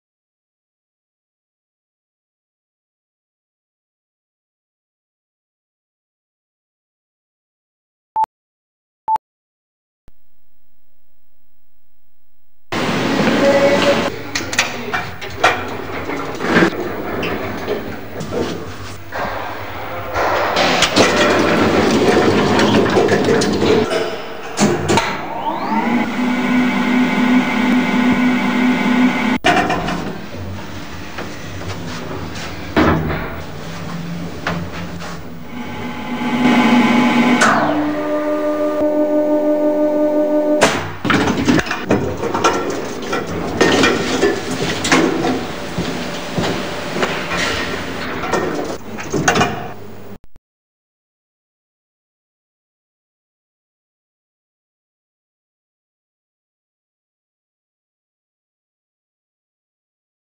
vidéo dans laquelle je prends l'ascenseur
vidéo dans laquelle
je prends l'ascenseur

video in which
I take the elevator

• 1998
• 1'16"
• Hi8 PAL
• Stereo
/ École Supérieure d'Art
d'Aix en Provence